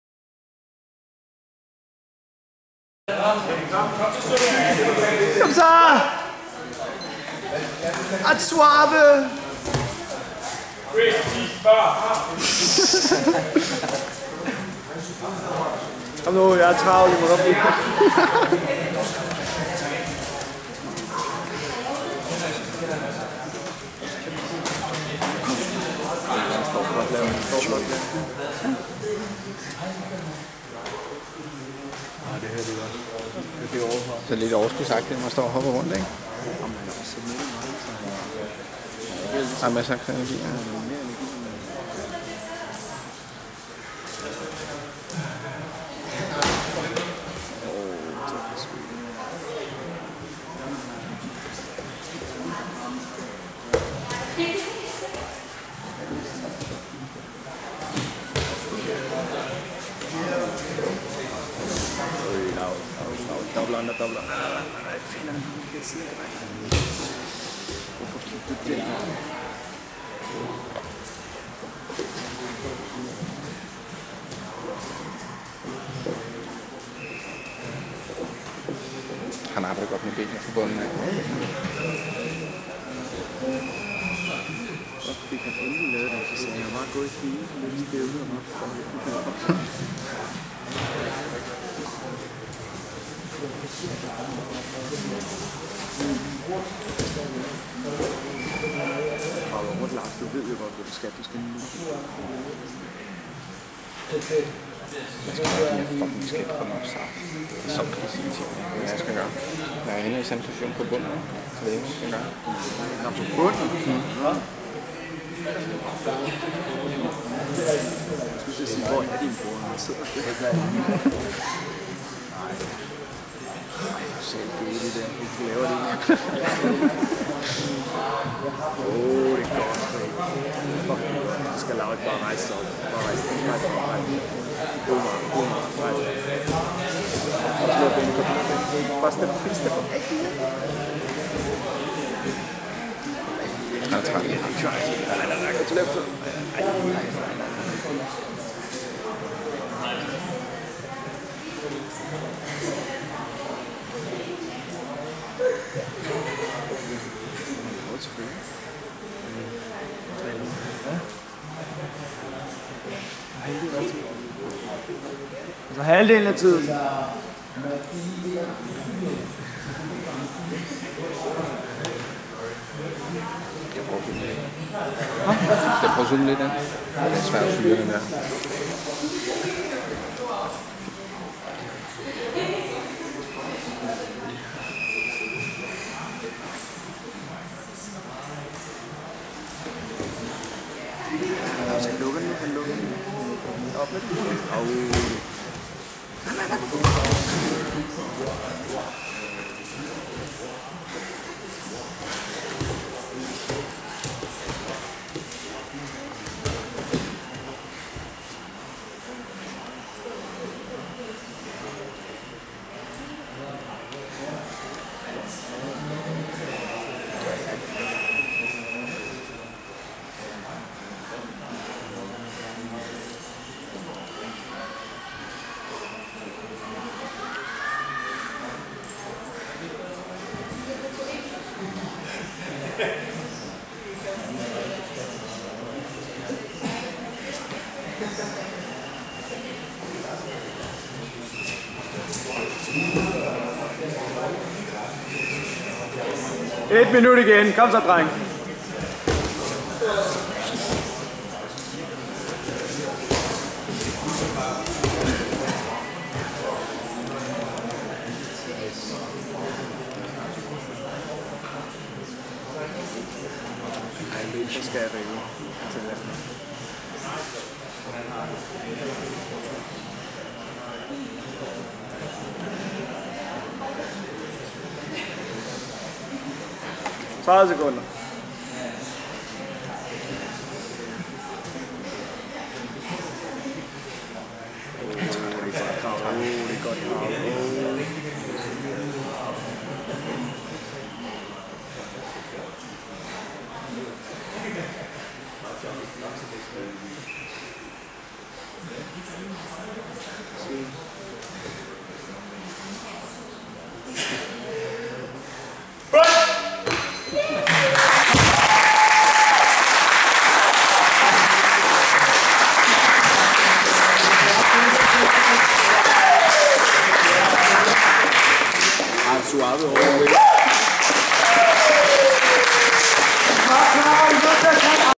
Laurits Tielbo CheckMat/Arte Suave vs Rico Birch CheckMat/Arte Suave
Danske Begynder  2011 konkurrence

Bracket A White Male Open Class Adult (18 years and up)